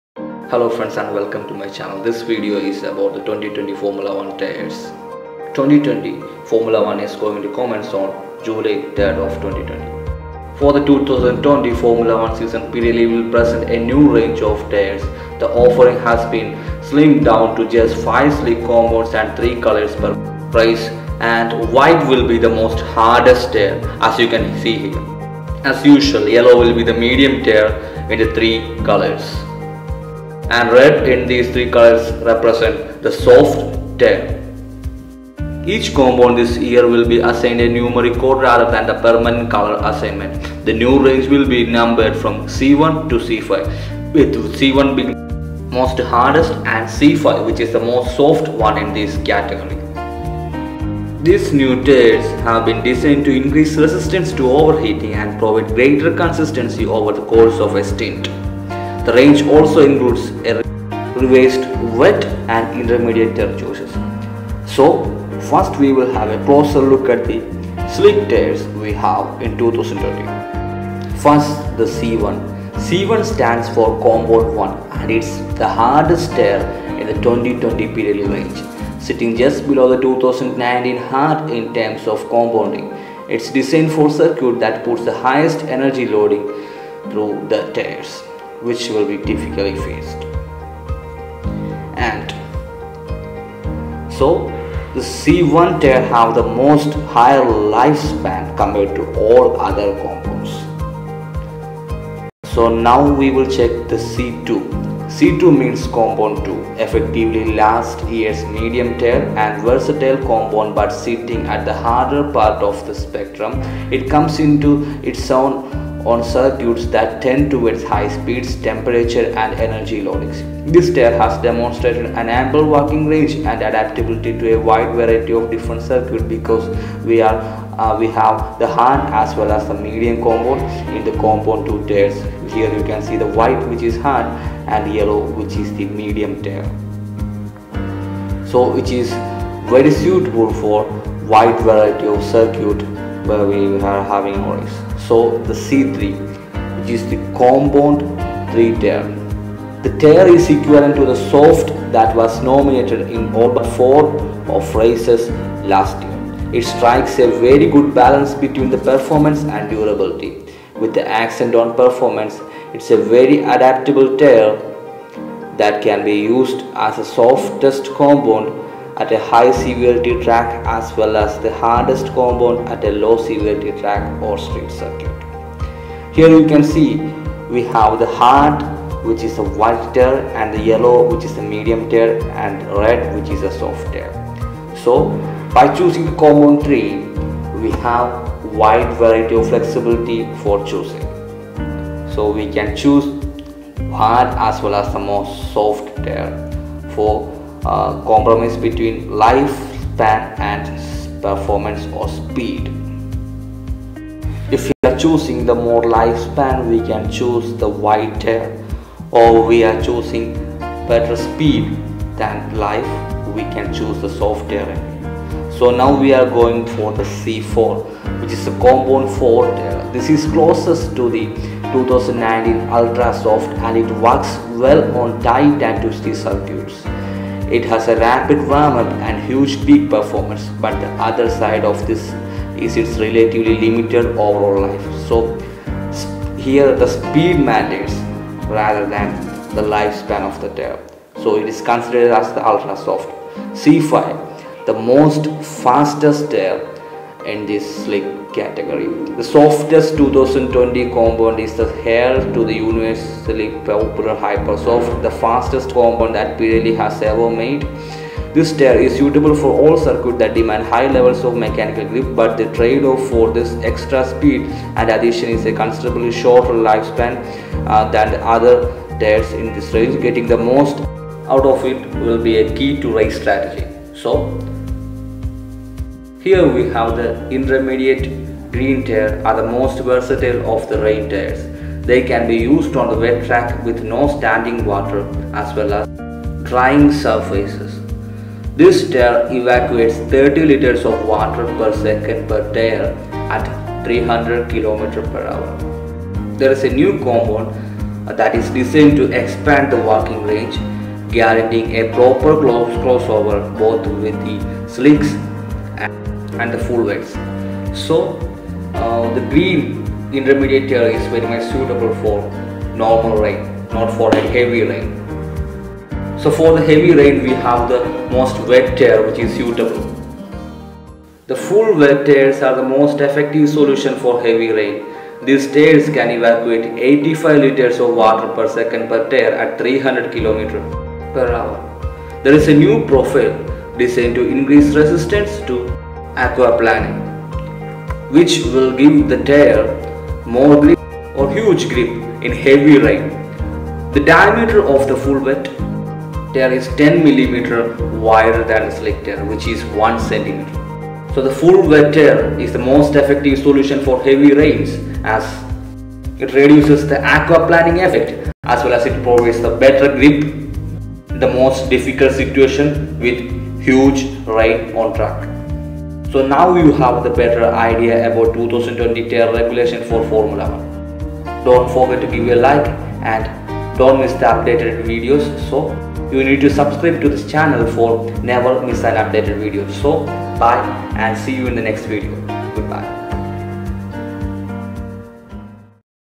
Pirelli Explains the 2020 Formula One tyre regulations, 2020 f1 tyres Explained
This video explains the new 2020 formula tyre regulations